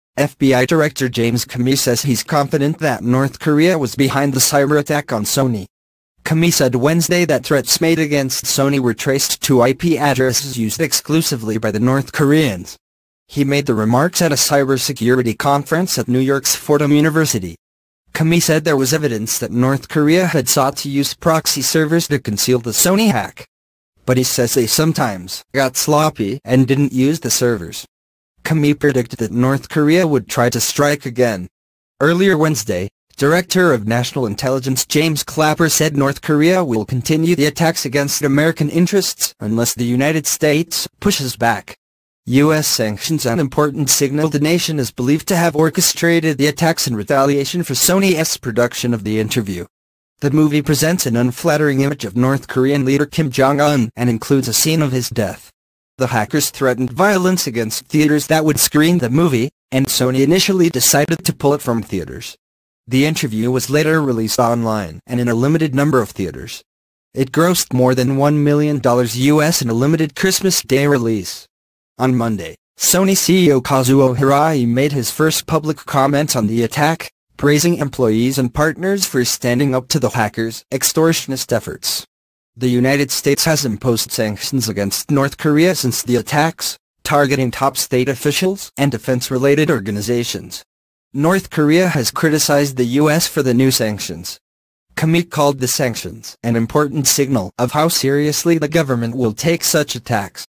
FBI Director James Comey says he's confident that North Korea was behind the cyberattack on Sony
Comey said Wednesday that threats made against Sony were traced to IP addresses used exclusively by the North Koreans. He made the remarks at a cybersecurity conference at New York's Fordham University.
Comey said there was evidence that North Korea had sought to use proxy servers to conceal the Sony hack. But he says they sometimes "got sloppy" and didn't use the servers.

Comey predicted that North Korea would try to strike again.

Earlier Wednesday, Director of National Intelligence James Clapper said North Korea will continue the attacks against American interests unless the United States "pushes back."

U.S. sanctions 'an important signal'
The nation is believed to have orchestrated the attacks in retaliation for Sony’s production of The Interview. The movie presents an unflattering image of North Korean leader Kim Jong-un and includes a scene of his death.
The hackers threatened violence against theatres that would screen the movie, and Sony initially decided to pull it from theatres.

The Interview was later released online and in a limited number of theatres. It grossed more than $1 million US in a limited Christmas Day release.

On Monday, Sony CEO Kazuo Hirai made his first public comments on the attack, praising employees and partners for standing up to the hackers' "extortionist efforts."

The United States has imposed sanctions against North Korea since the attacks, targeting top state officials and defence-related organizations.

North Korea has criticized the U.S. for the new sanctions. Comey called the sanctions "an important signal" of how seriously the government will take such attacks.